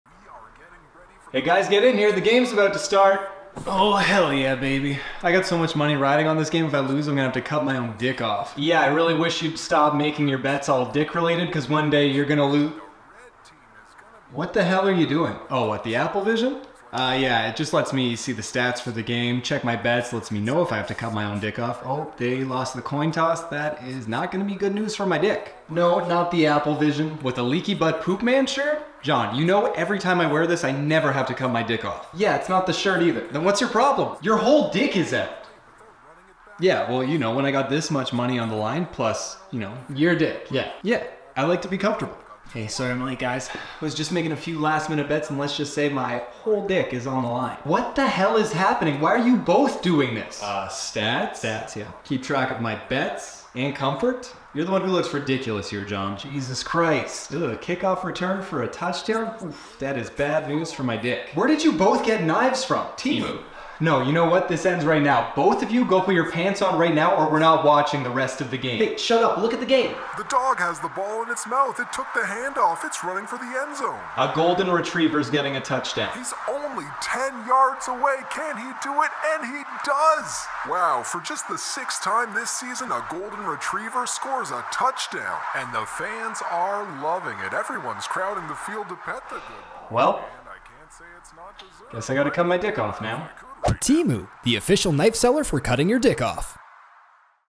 THE DANGERS OF GAMBLING
Three completely different guys get together to watch the big game and put some money down on it.